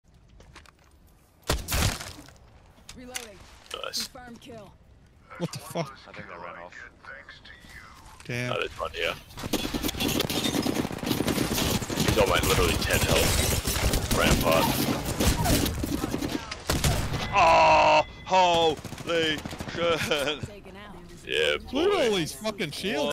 Triple the Filth
Savage Revenant ult rush deletes unwitting squad.